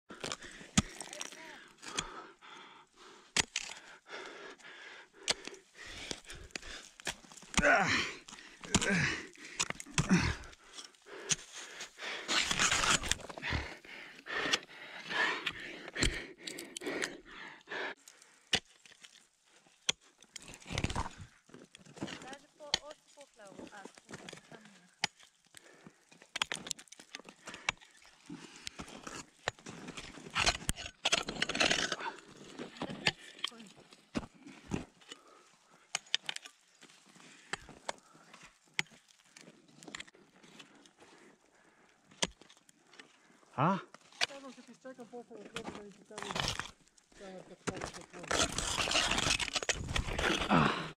Ice Climbing FALL caught on camera
Hi, in this video you can see my ice climbing fall. I had 2 times luck before but in the end one bad move could cost me a serious injury. I wasn't hurt bad from this fall and continued climbing afterwards. Bye!